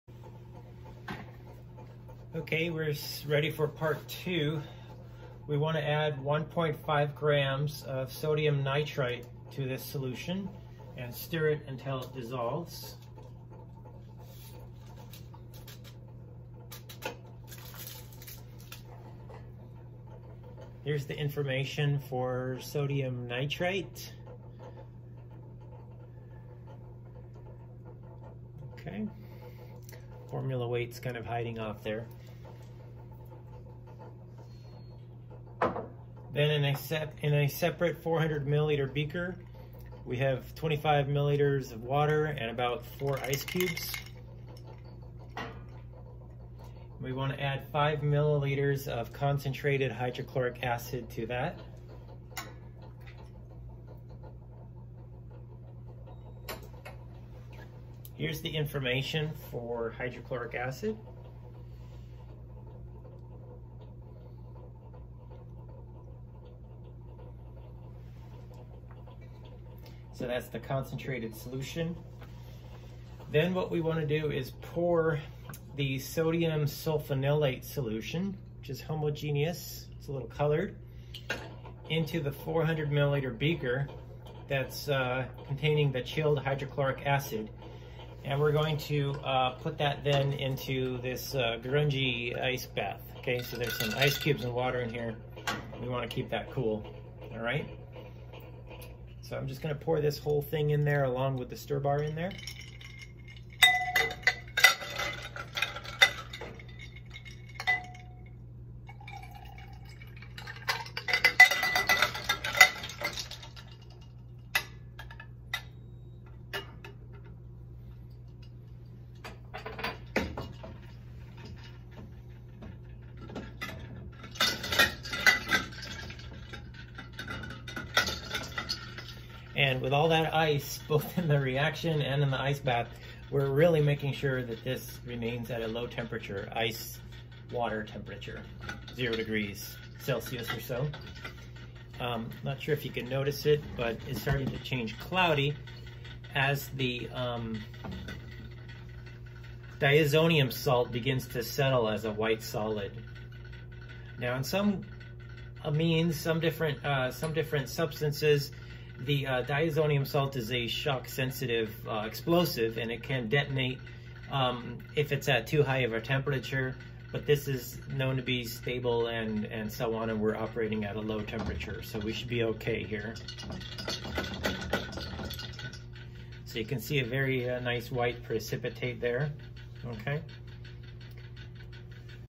Lab 2 Methyl Orange Part 2
This laboratory experiment involves the synthesis of methyl orange. This video is part of the playlist for this experiment.